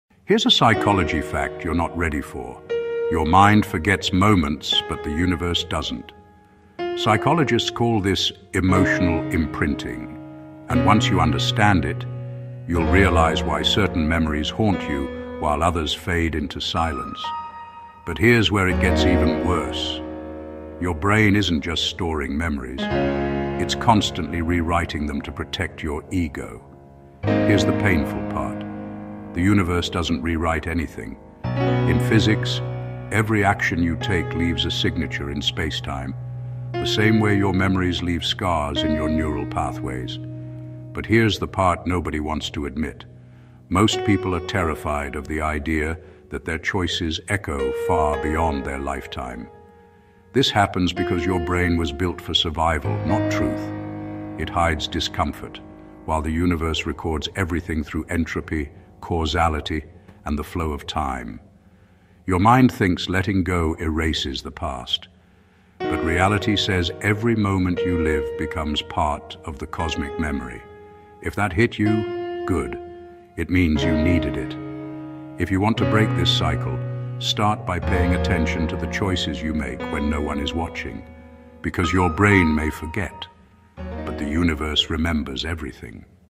The Universe Remembers What You’d Rather Forget
Here’s a psychology fact you’re not ready for: your mind forgets moments, but the universe doesn’t. This 50s YouTube Short blends psychology, neuroscience and physics to explain “emotional imprinting,” memory rewriting, and why choices echo in spacetime. Perfect for curious psychology fans — educational, philosophical background music; yellow comic-box subtitles; male heavy Northern English narration. Watch to confront how memory, entropy and causality shape your life and learn the one change that breaks the cycle: pay attention when no one’s watching.